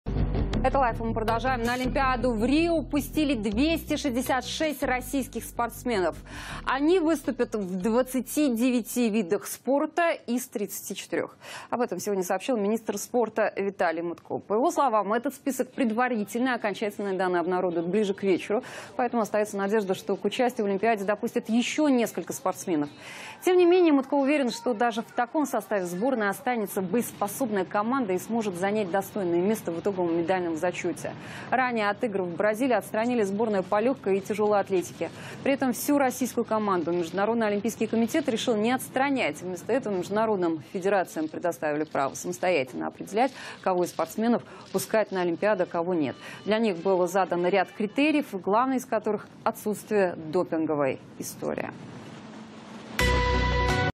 Россияне смогут выступить на Олимпиаде в Рио-де-Жанейро в 29 видах спорта
Виталий Мутко: На данный момент допуск на участие в играх получили 266 атлетов